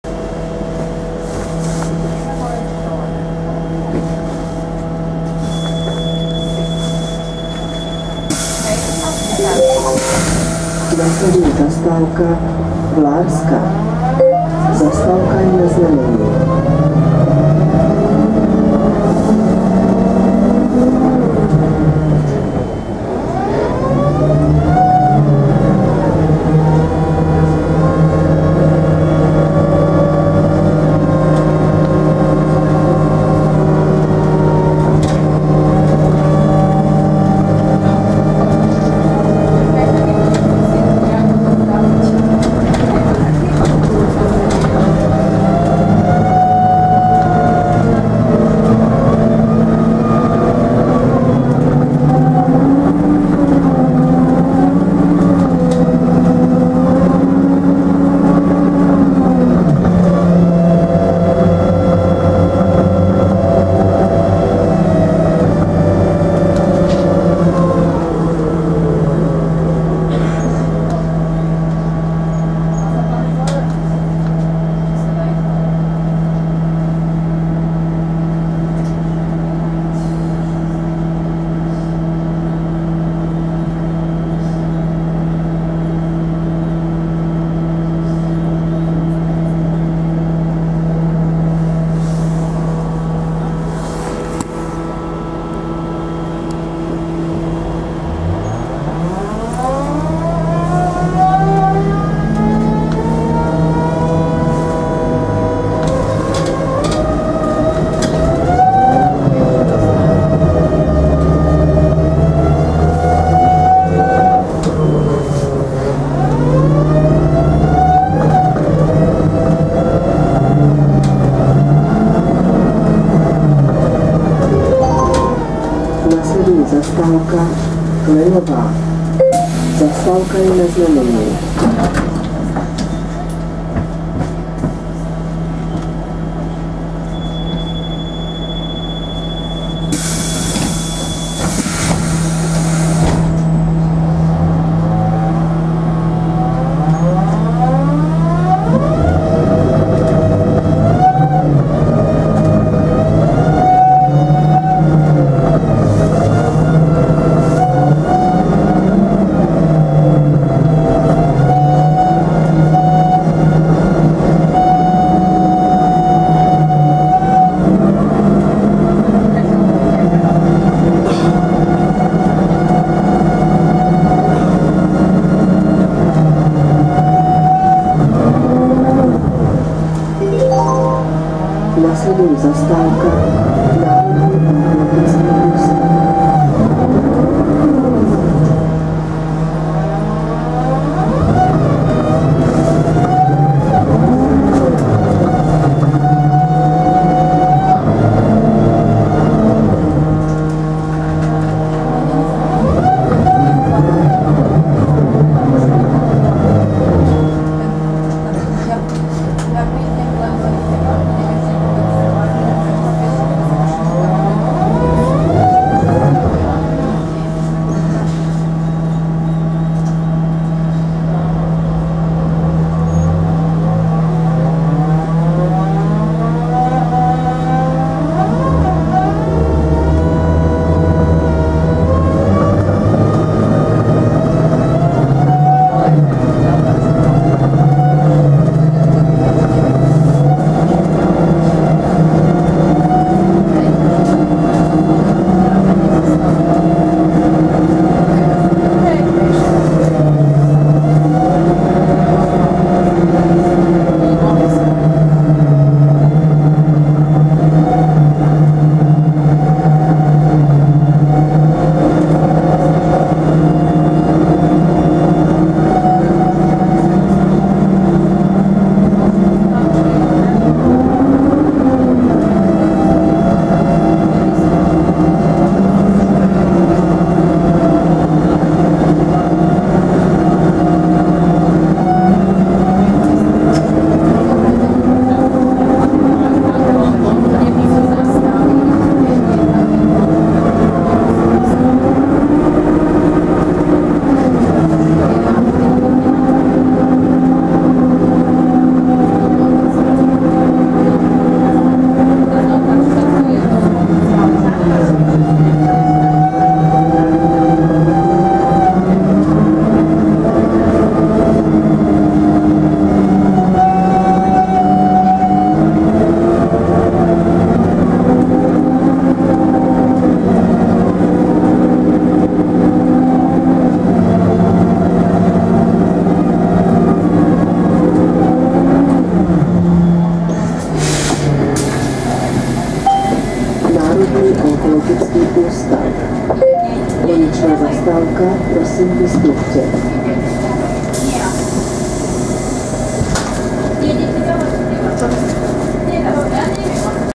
5.1.2015 - Škoda 14 Tr 08 6241 (* 31.12.1987 † 16.1.2015)
Škoda 14 Tr 08 6241 počas snehovej kalamity sa snaží vyšplhať z Magurskej k Národnému onkologickému ústavu. Podarilo sa jej to len po zastávku Klenová.